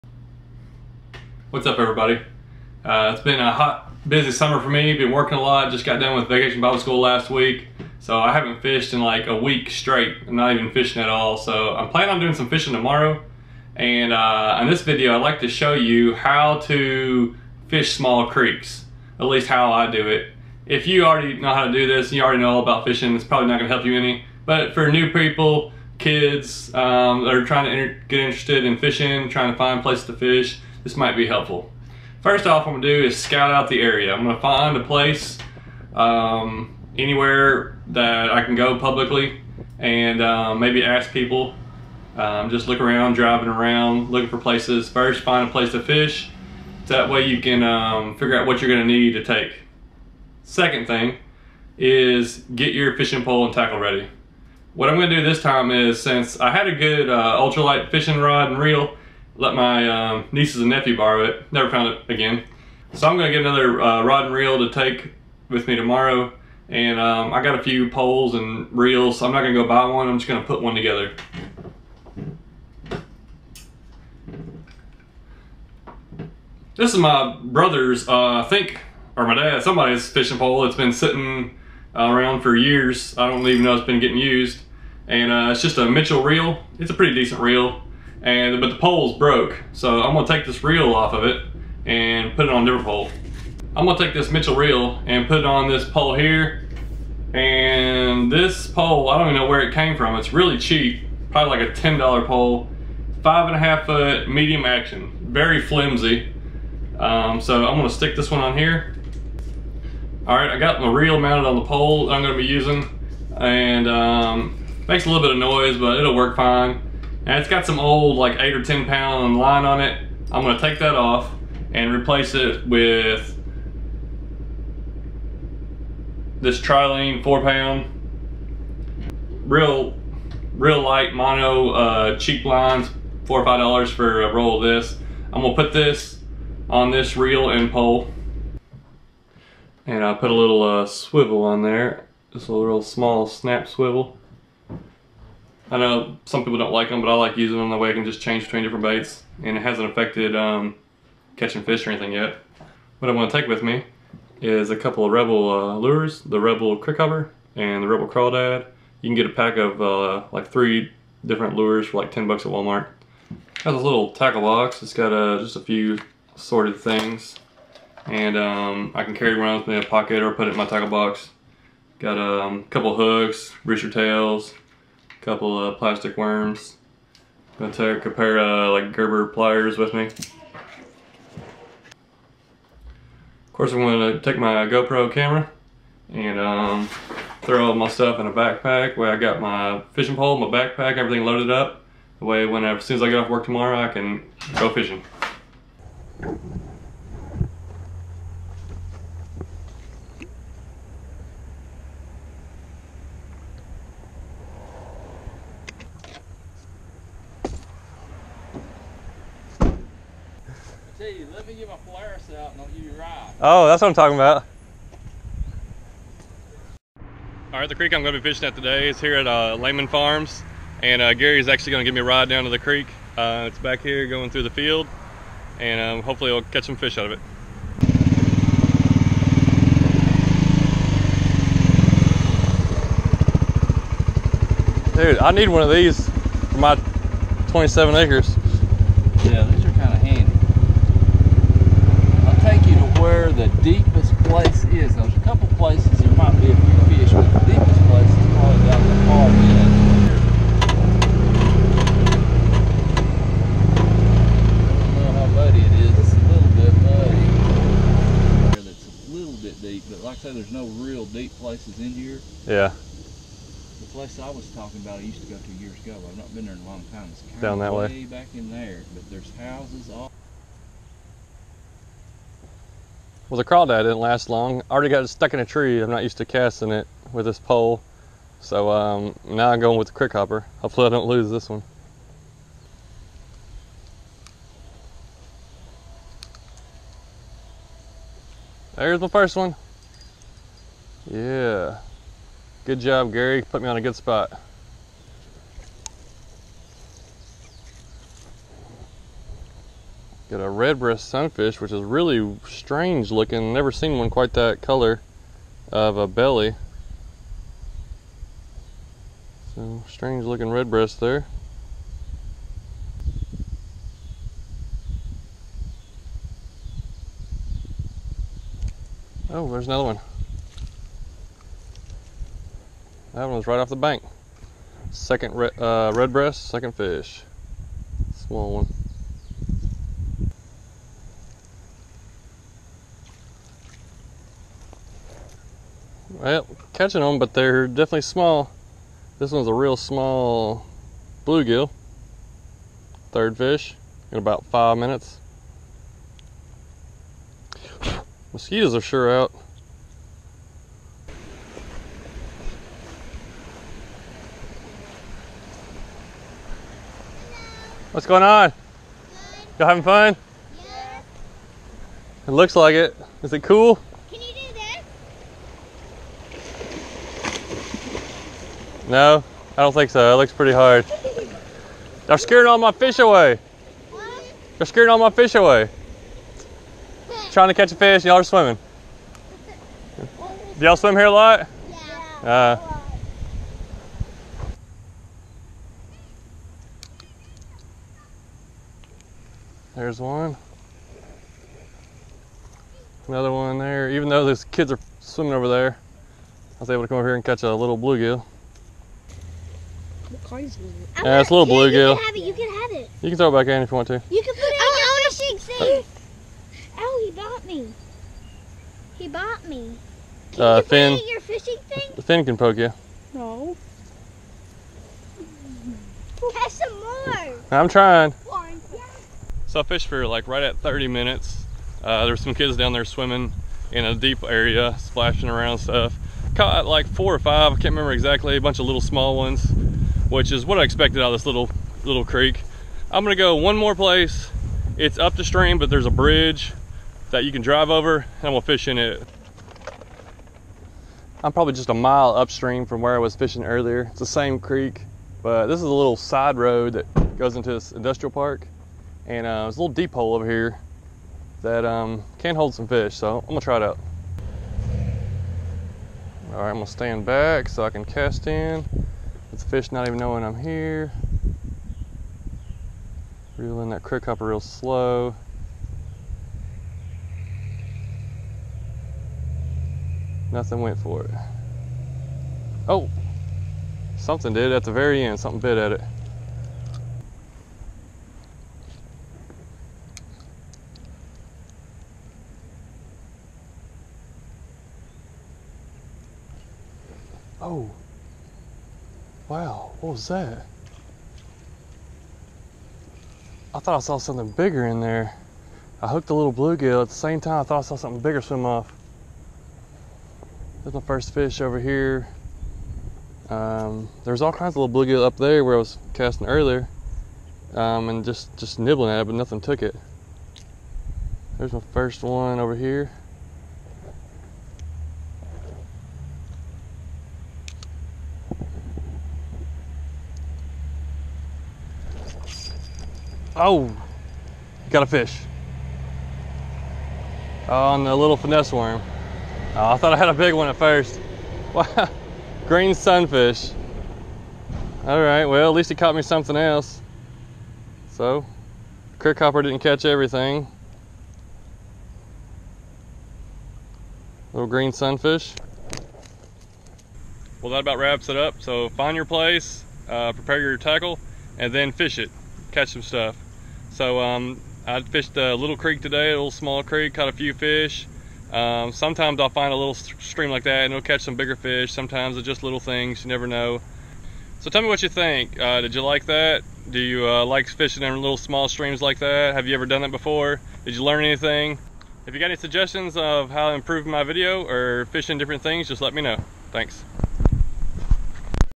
How I Find and Fish Small Creeks
This is just a simple video showing how I go about fishing a small creek. Hope it can help someone if they are looking to start fishing or find a creek to fish in.